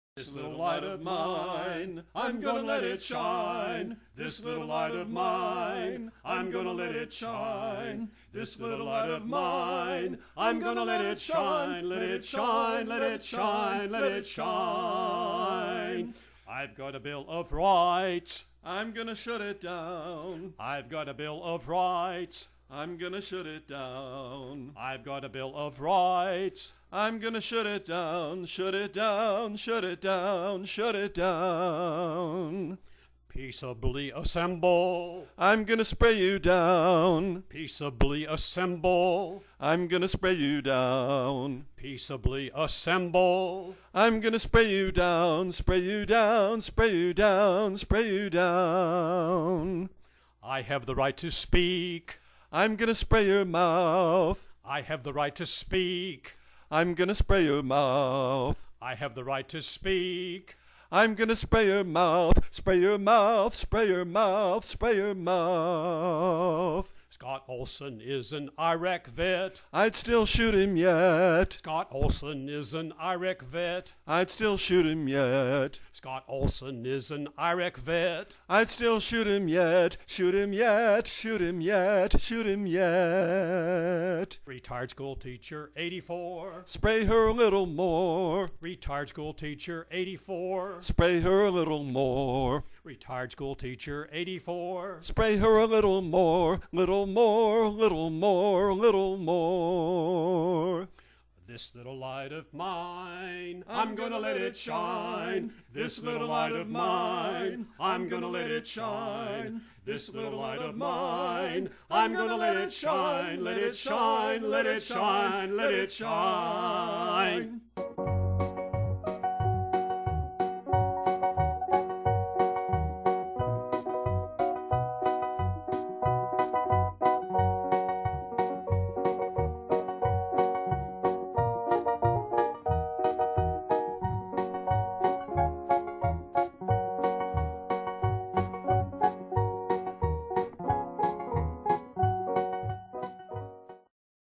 A Little Light On Occupy Wall Street and Police Brutality
This video uses pictures and song to contrast the message of Occupy Wall Street with Police Brutality.